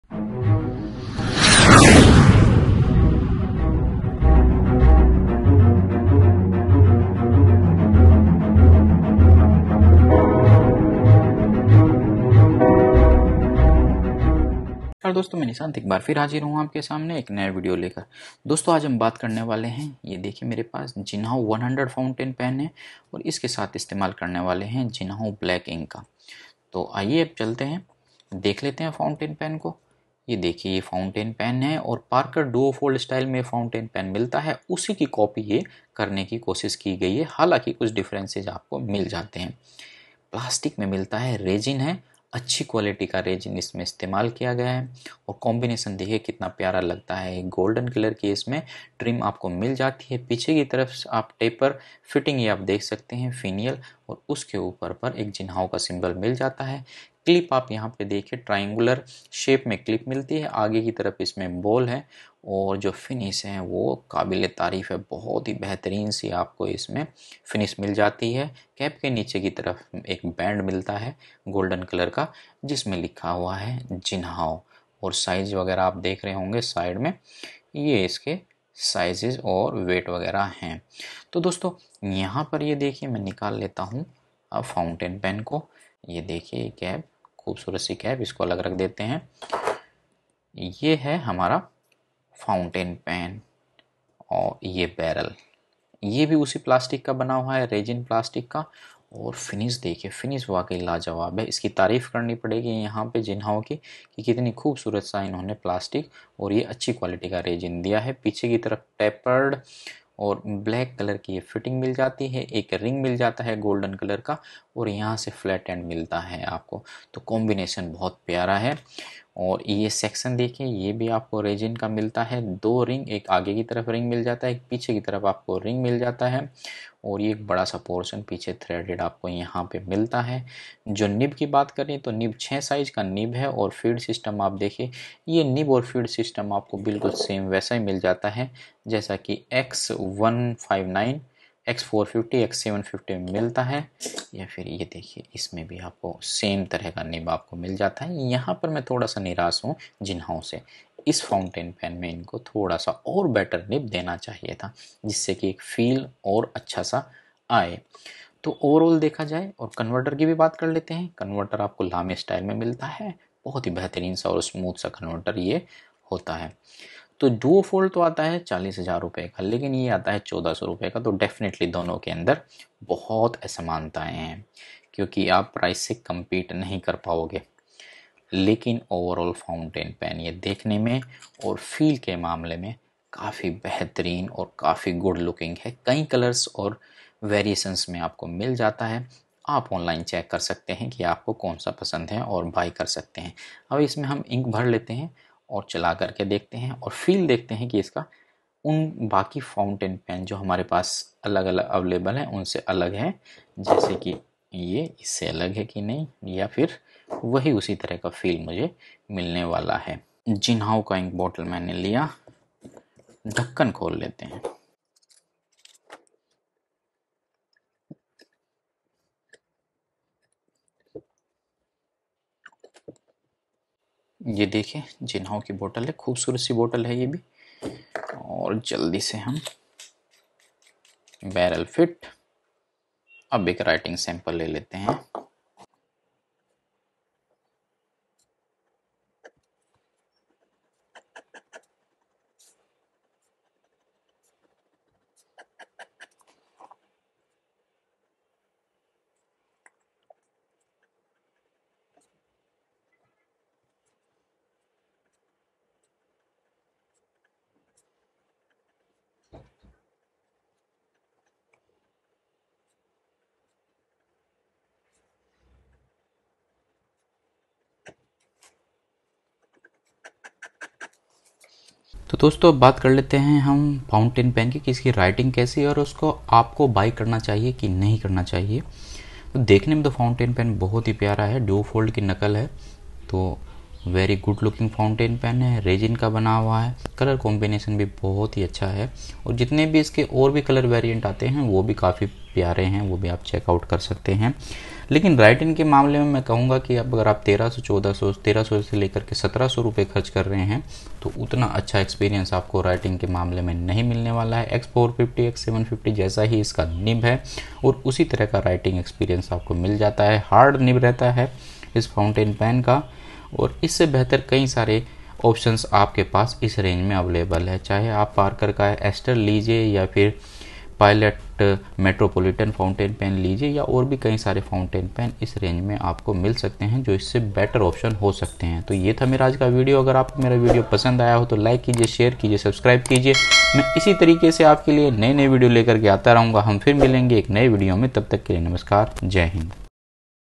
Jinhao 100 Fountain Pen Review | Is it worth for 1300 rs |FOUNTAIN PEN REVIEWS|फाउंटेन पेन|इंक पेन
Unique Design- Excellent resin material, beautiful barrel with gold trim is very beautiful.

For sale
 jinhao 100 centinental fp✒️
Fine nib with converter
Rs1150/ each all includes 🙂

FOUNTAIN PEN REVIEWS
AAPKI UNBOXING